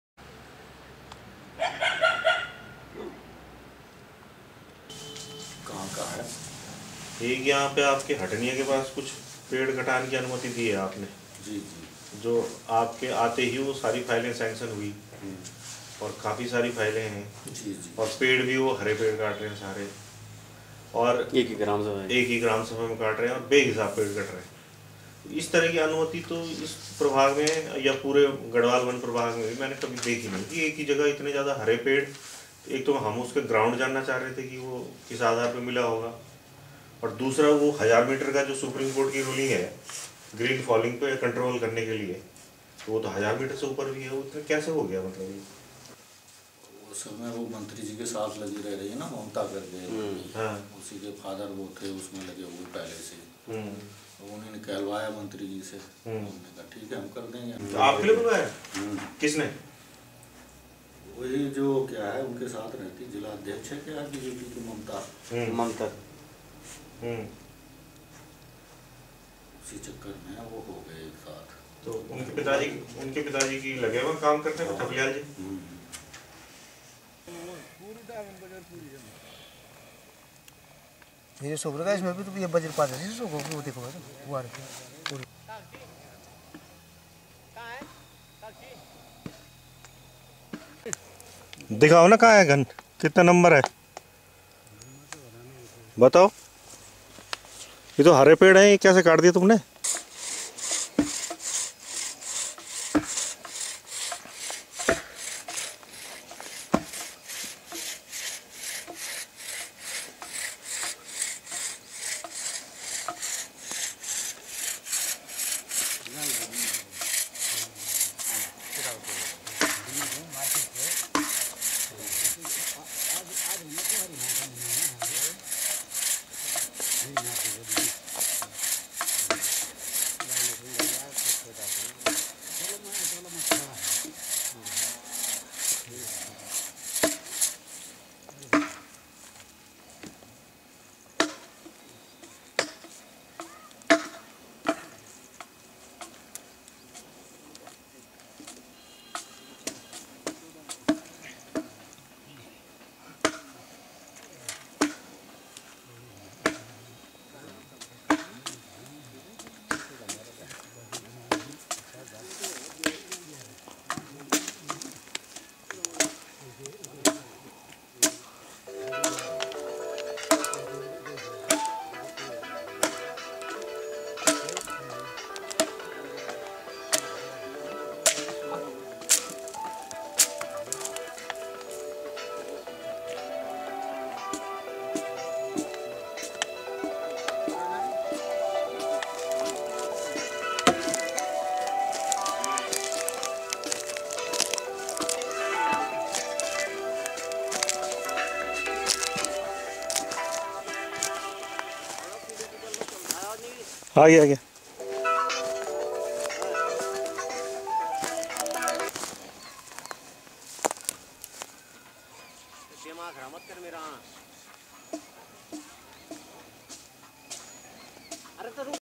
"भाजपा कार्यकर्ता" या "वन माफिया " ?
"जागो उत्तराखण्ड" बिग  ब्रेकिंग

उत्तराखण्ड की ज़ीरो टॉलरेंस वाली सरकार के भाजपा पदाधिकारियों का कारनामा..
 वन मन्त्री  हरक सिंह रावत की आड़ में चलवा दी सैकड़ों हरे पेड़ों पर आरियाँ #

 उत्तराखण्ड में जीरो टॉलरेंस वाली भारतीय जनता पार्टी के पदाधिकारी किस तरह से भ्रस्टाचार और अवैध धंधों में लिप्त हैं इसका खुलासा लैंसडाउन वन प्रभाग  के डीएफ़ओ सन्त राम की ज़ुबानी हुआ है ,सन्तराम ने ऑन कैमरा बताया कि कोटद्वार में भारतीय जनता पार्टी की महिला मोर्चा की अध्यक्ष  ममता थपलियाल देवरानी ने अपने लकड़ी कारोबारी पिता को कोटद्वार -पौड़ी राष्ट्रीय राजमार्ग से लगे हटनिया के पास नाप भूमि में चीड़ के दर्जनों हरे पेड़ो के कटान की स्वीकृति वन मंत्री से कहलवा कर दिलवा दी है ,हैरानी की बात यह है कि यँहा पेड़ कटान की दर्जनों फाइलें स्वीकृत हैं और अब तक यंहा पर सैकड़ों हरे पेड़ों पर आरी चल चुकी है जो कि वन अधिनियम की धाराओं का स्पष्ट उल्लंघन है और माननीय सर्वोच्च न्यायालय और एन जी टी के 1000 मीटर से ऊपर हरे पेड़ों के कटान पर प्रतिबन्ध का भी मज़ाक है